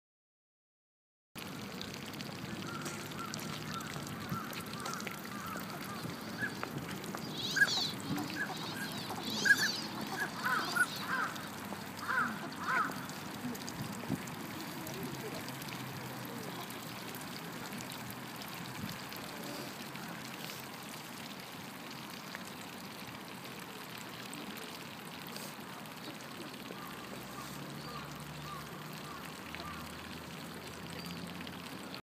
Duck moving strangely 謎な動きをするカモたち。
＠不忍池にて。
ホバリングしているかのような動きで水面のくちばしで震わせてるような、水を飲んでいるような。何してんだろ... シャバシャバ聞こえるおとは、くちばしで水が震わされている音でありんす。